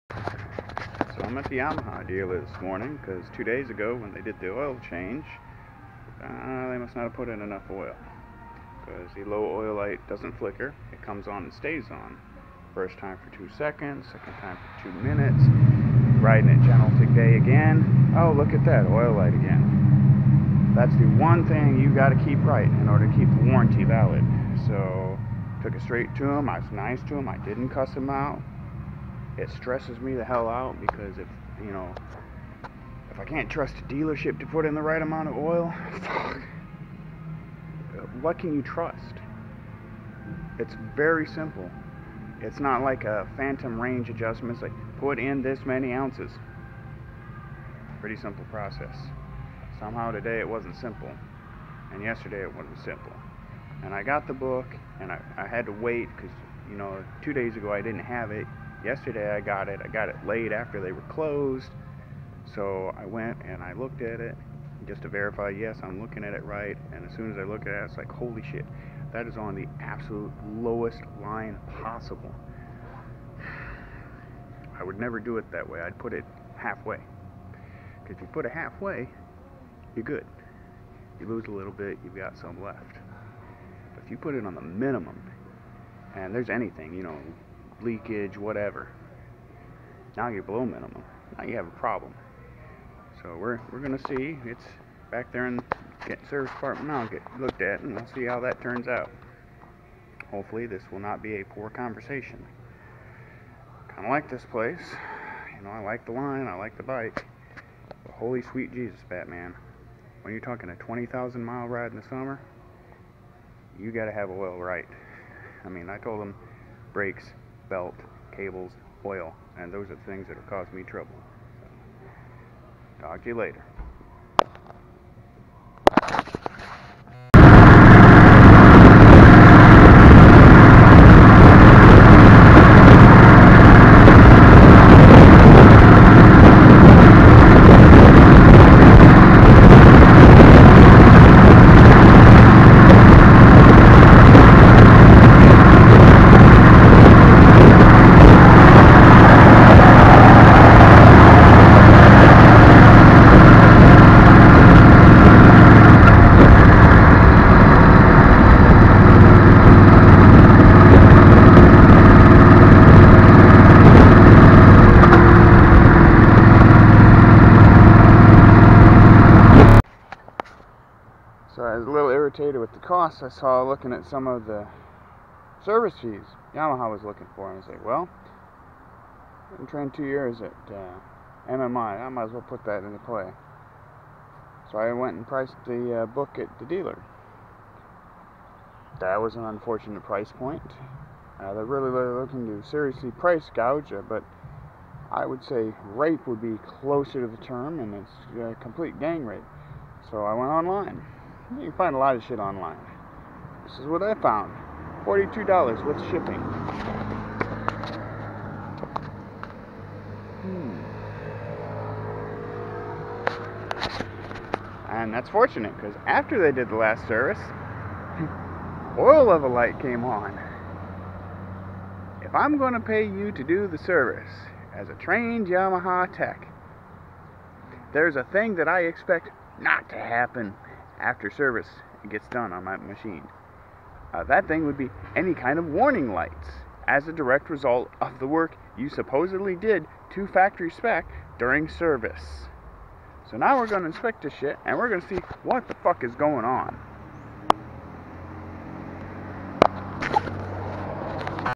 Dealer service FAIL, having fun AFTER dealer service 2017
Sometimes you get busy and you take your motorcycle to the dealer for service. I do that occasionally. My expectations are simple. I pay them money and they follow the maintenance standards laid out in the service repair manual.